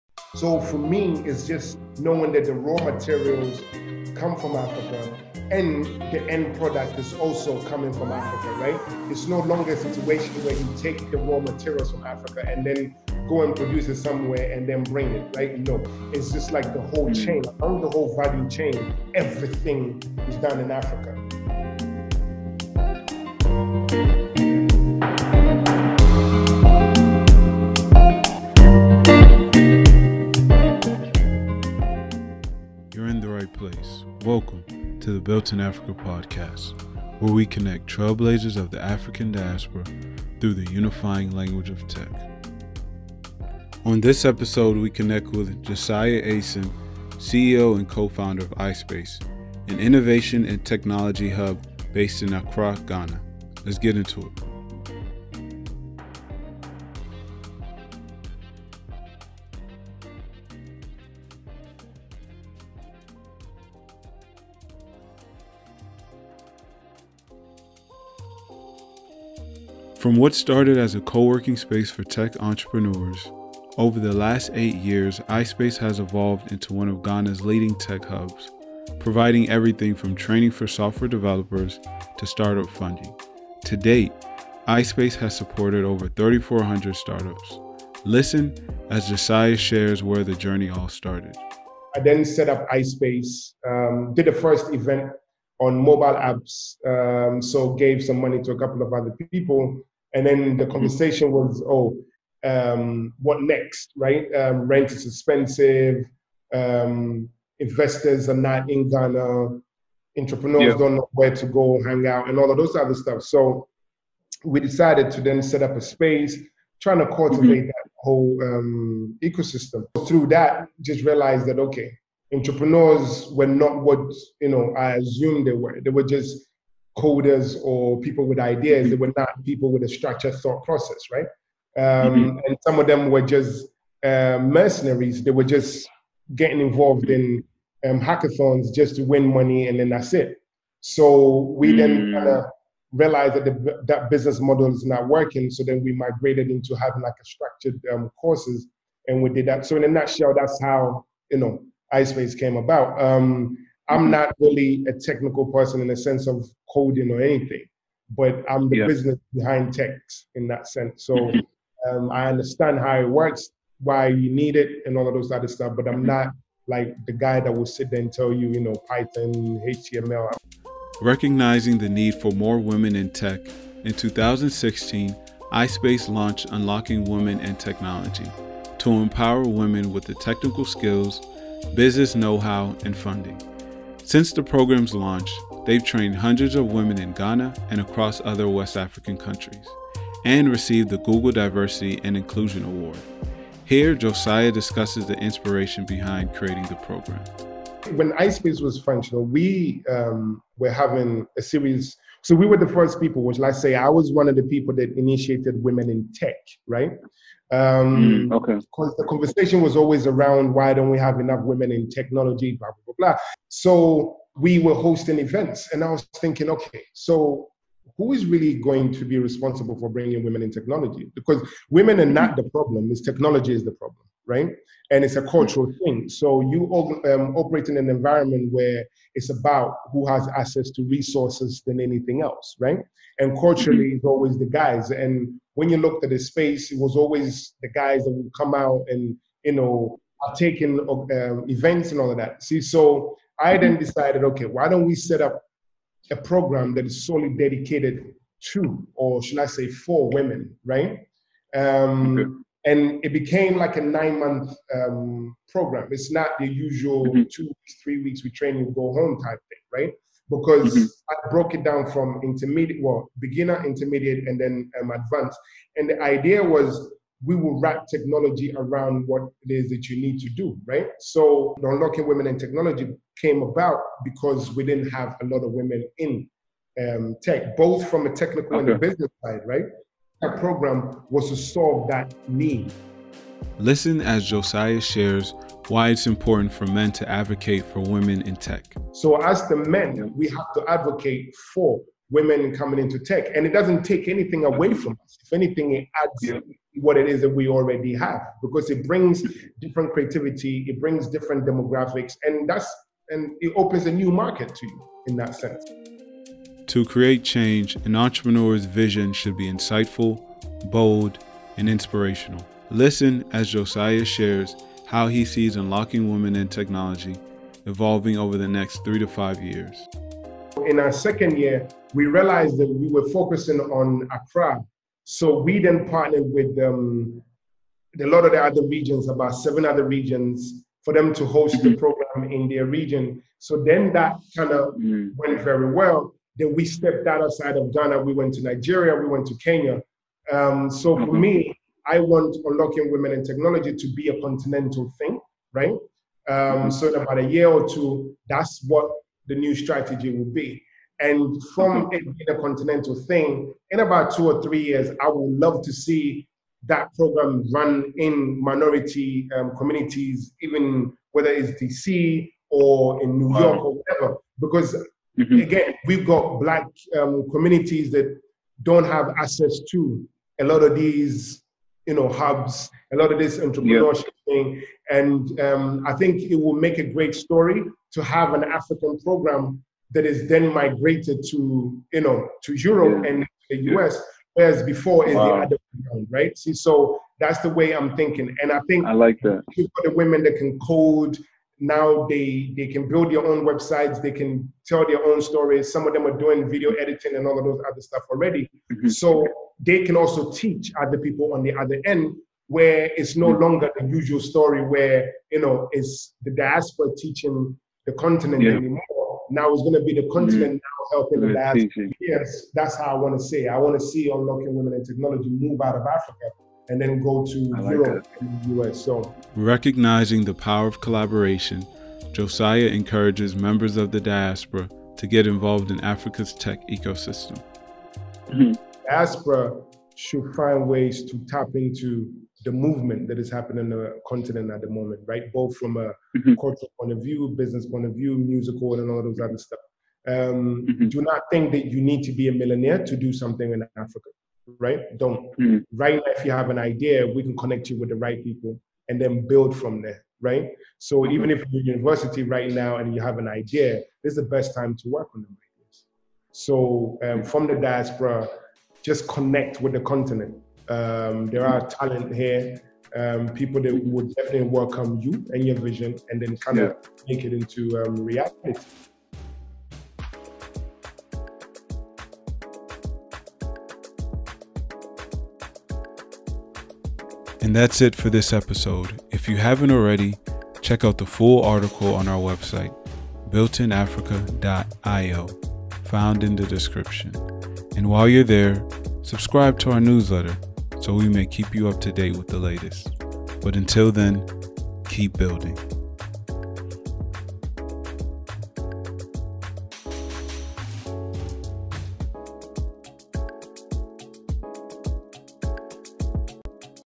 Unlocking Women and Technology: Josiah Eyison CEO of iSpace Ghana
Unlocking Women and Technology, a product of iSpace Ghana, empowers women through tech and business development training. We spoke with Josiah Eyison, Co-Founder/CEO of iSpace Foundation, about UWAT, why men are equally responsible for women in tech advocacy, and how the diaspora can contribute to Africa's development.

Timestamps:
0:00 – What does Built In Africa mean to you?
1:10 – Josiah share’s what led him to create iSpace Foundation.
2:50 – The story behind starting Unlocking Women and Technology.
5:06 – How UWAT will evolve over the next 3-5 years.
7:00 – How the diaspora can get involved in Africa’s tech ecosystem.